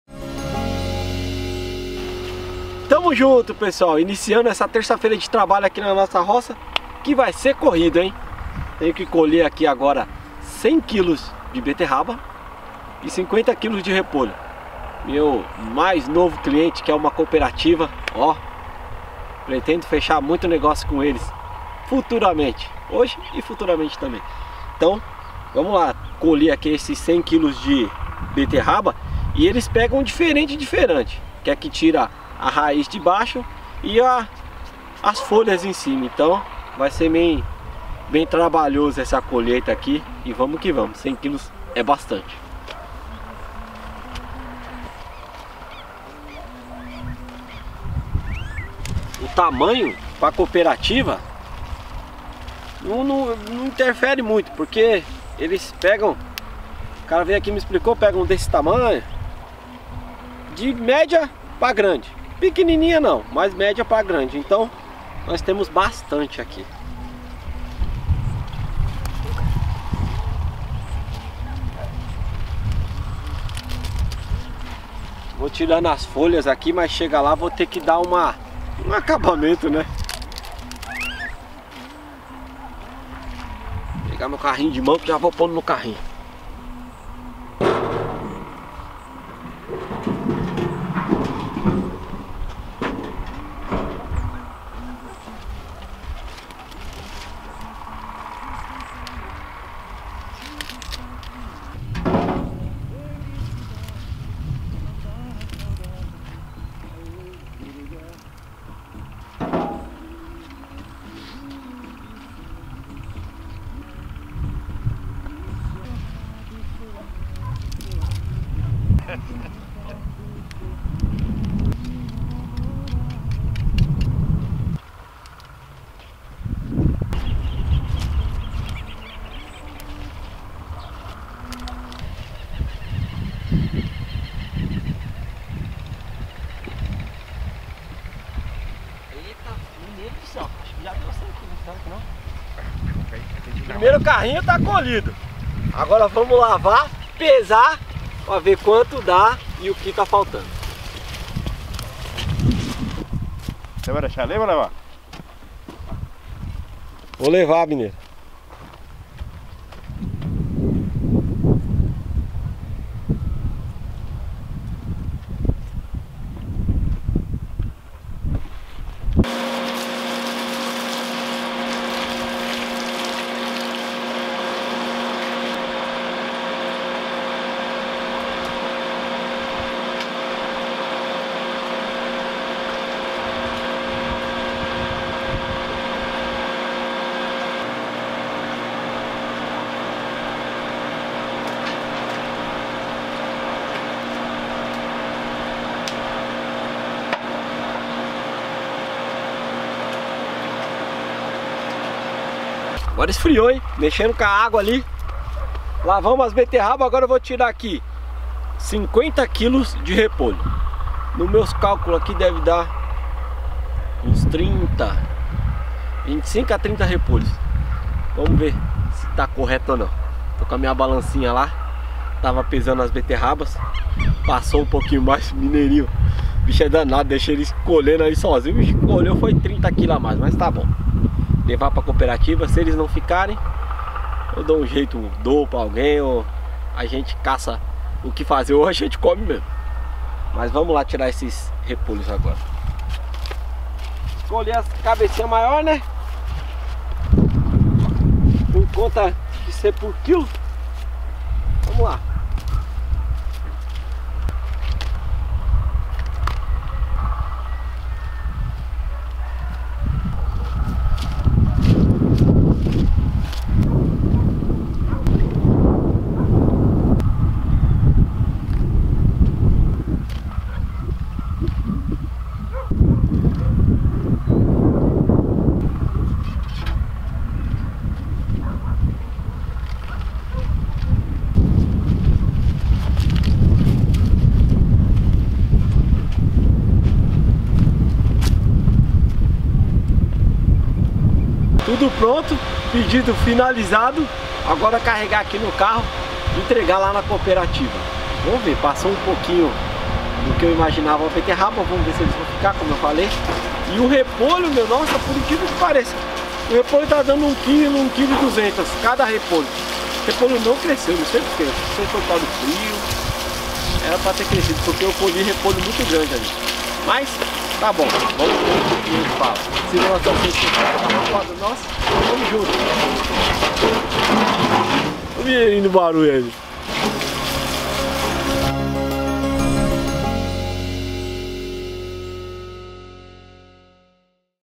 VENDI 150 Kg PARA COOPERATIVA | AGRICULTURA FAMILIAR
Hoje o dia foi tão corrido mais tão corrido que o vídeo saiu só agora  21:25.
Marcos Meira fez a sua primeira venda na cooperativa, e espera vender muito mais.
Na verdade ele vendeu um total de 175 kg no total.